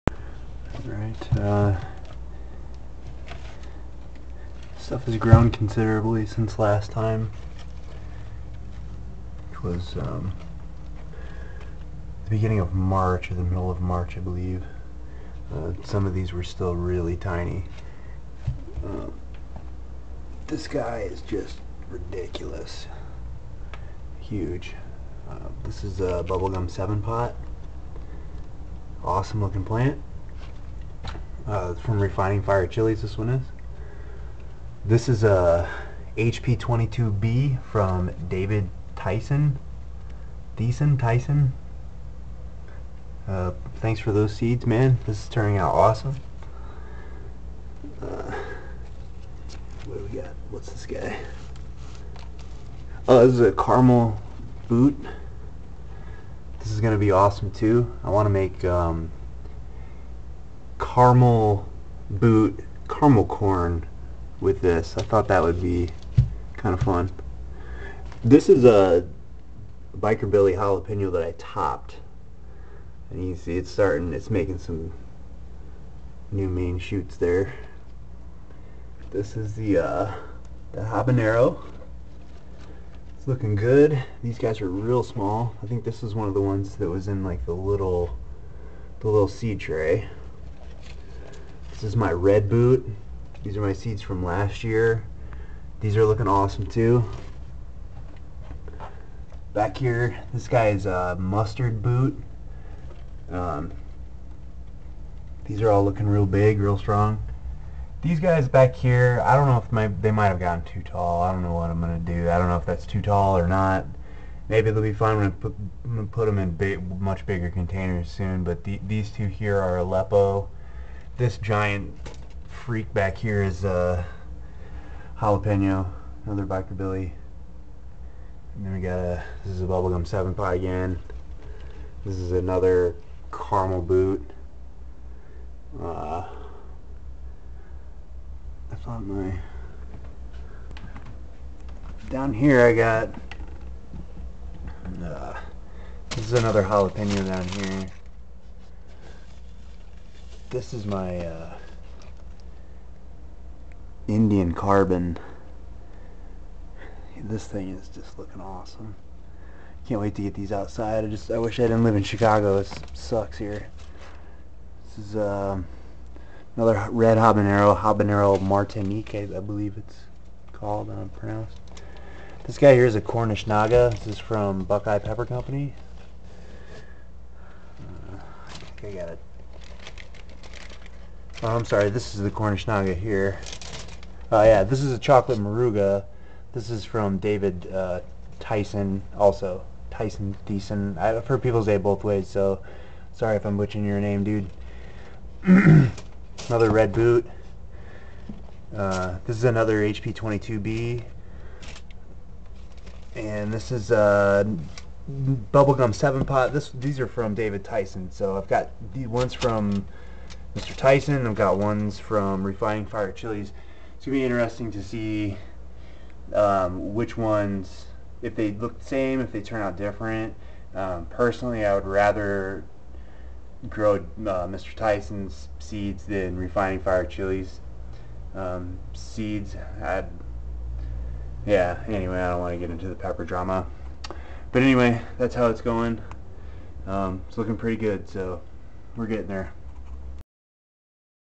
superhot pepper garden 2015 UPDATE!!
my plants have grown a large amount since the last video.  i am still a novice at this so thanks in advance for any help!  thanks to david theisen, brad bishop, and buckeye pepper company.